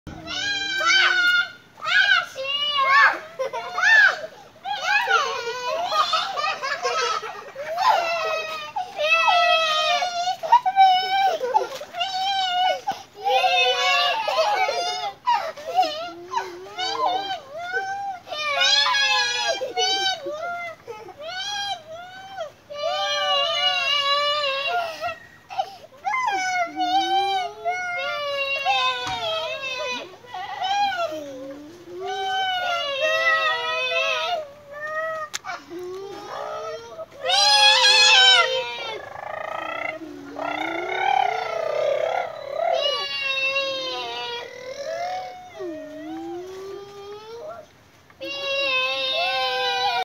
Jhumping baby...ছোট্ট বাচ্চারা যে ভাবে গাছে ওঠে দোল খেলে . the way the little kids play in the trees
Jhumping baby...Jhumping baby...ছোট্ট বাচ্চারা যে ভাবে গাছে ওঠে দোল খেলে . the way the little kids play in the trees আমাদের গাছ ঋড়ে ভেঙ্গে যাই তার ছোটরা যে ভাবে দোল খাই The way our little children break down in our trees